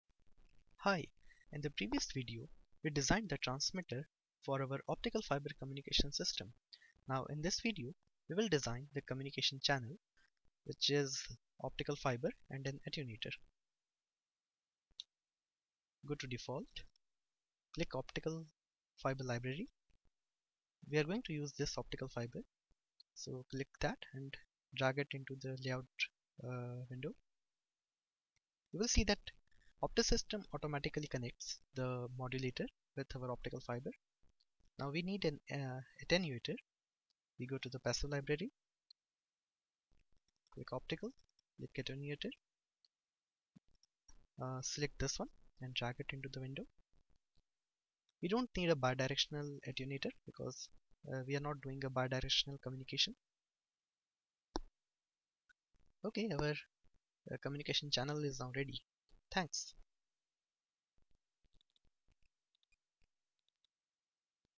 Introduction to Optisystem - communication channel
Optisystem is a powerful software tool for designing optical communication system. This video is a general introduction to Optisystem and deals with designing a communication channel for optical fiber communication system.